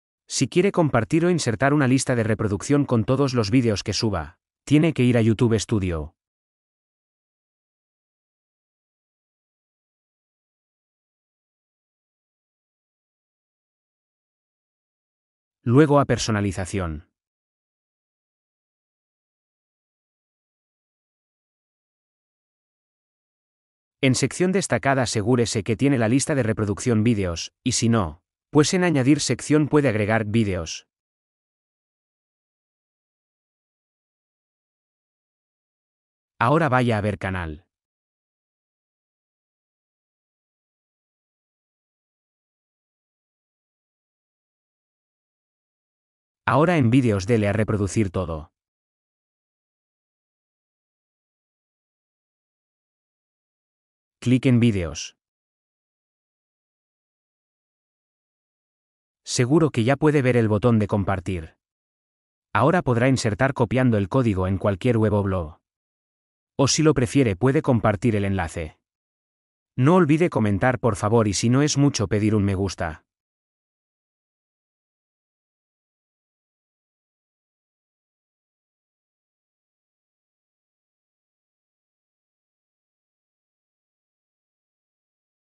Cómo insertar o compartir listas de reproducción con todos los vídeos que subes a YouTube
Tutorial_de compartir o insertar una lista de reproducción con todos los vídeos que suba a YouTube.

1. Accede a YouTube Studio.
2. En el menú de la izquierda, haz clic en "Personalización".
3. En la sección "Sección destacada", asegúrate de que tienes la lista de reproducción "Vídeos". Si no la tienes, puedes añadirla haciendo clic en "Añadir sección" y seleccionando "Vídeos".
4. Haz clic en "Ver canal".
5. En la sección "Vídeos", haz clic en "Reproducir todo".
6. Haz clic en la lista de reproducción "Vídeos".
7. Ahora deberías ver el botón de compartir. Puedes insertar el código copiando y pegando en cualquier web o blog, o compartir el enlace.

**Consejos:**

* Si quieres que la lista de reproducción se reproduzca en orden aleatorio, haz clic en el icono de la ruleta de la lista de reproducción.
* Si quieres que la lista de reproducción se reproduzca en un bucle, haz clic en el icono de repetición de la lista de reproducción.

**Extra:**

Para que la lista de reproducción se vea mejor, puedes personalizarla añadiendo una imagen de portada y una descripción.

Espero que esto te ayude a compartir o insertar tus vídeos de YouTube.

🎯 Dale👍y_comenta para apoyarme. ¡Gracias!